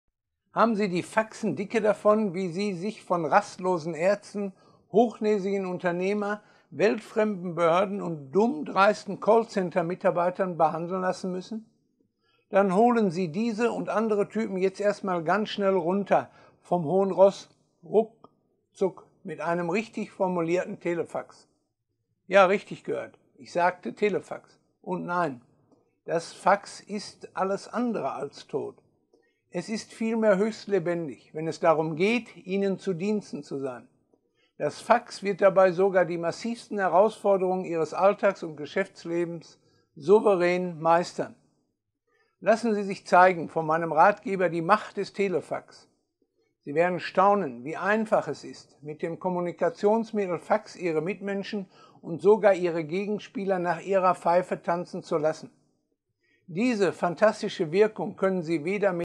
Schluss mit dem Verhungern in der Warteschleife!
Haben Sie nicht auch den Eindruck: Je schneller unsere Zeit wird, desto länger dauert es, bis Sie mit den richtigen Leuten telefonieren können?
Also, vergessen Sie WhatsApp & Co., wenn Sie mehr für sich erreichen wollen. Entdecken Sie ein Kommunikationsmittel, das völlig zu Unrecht als »tot« bezeichnet wird: Nutzen Sie »Die Macht des Telefax«! Denn ein Fax schlägt Brief, Handy & E-Mails um Längen!

Dabei gibt es nur drei Dinge zu beachten:
1.) Vergessen Sie nie: Probleme sind Deine Probleme! Diese Weisheit stammt von dem indischen Philosophen „Rajneesh“ Chandra Mohan Jain.
2.) Glaube vor allem an dich selbst und an die Kraft, die in dir wohnt. – Dieser Ratschlag stammt von der deutschen Schriftstellerin Karin Heinrich.
3.) Die Lösungen werden sich ergeben, sobald Sie ins Handeln gekommen sind.